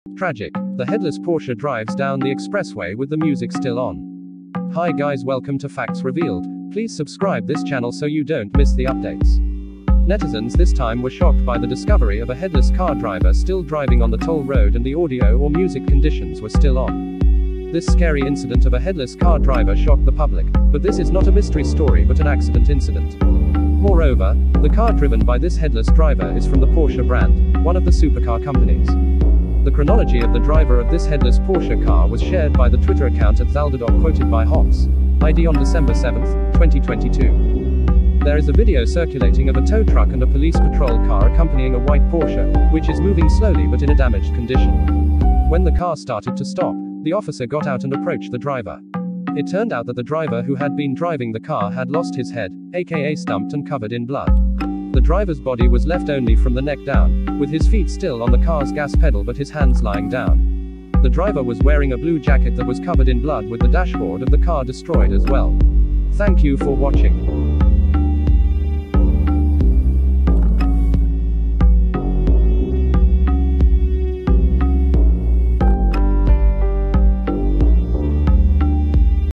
Tragic! The headless Porsche drives down the expressway with the music still on. PORSCHE A14 CASE
Tragic! The headless Porsche drives down the expressway with the music still on.

Netizens this time were shocked by the discovery of a headless car driver still driving on the toll road and the audio or music conditions were still on.

This scary incident of a headless car driver shocked the public, but this is not a mystery story but an accident incident.

Moreover, the car driven by this headless driver is from the Porsche brand, one of the supercar companies.

The chronology of the driver of this headless Porsche car was shared by the Twitter account @zaldidot quoted by Hops.ID on December 7,2022

There is a video circulating of a tow truck and a police patrol car accompanying a white Porsche, which is moving slowly but in a damaged condition.

When the car started to stop, the officer got out and approached the driver. It turned out that the driver who had been driving the car had lost his head, aka stumped and covered in blood.

The driver’s body was left only from the neck down, with his feet still on the car’s gas pedal but his hands lying down.

The driver was wearing a blue jacket that was covered in blood with the dashboard of the car destroyed as well.